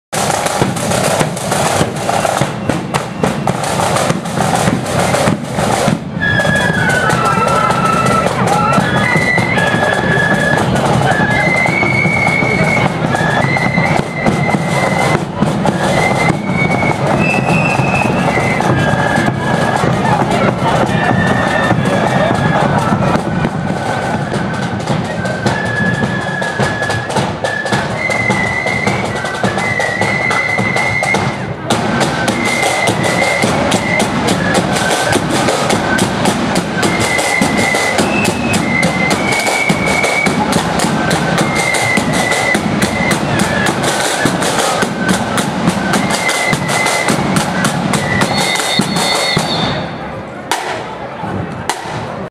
Ballyrea Boyne Defenders @ Pride of Knockmore 09/05/14
Ballyrea Boyne Defenders On the March in Lisburn for Pride of Knockmore annual parade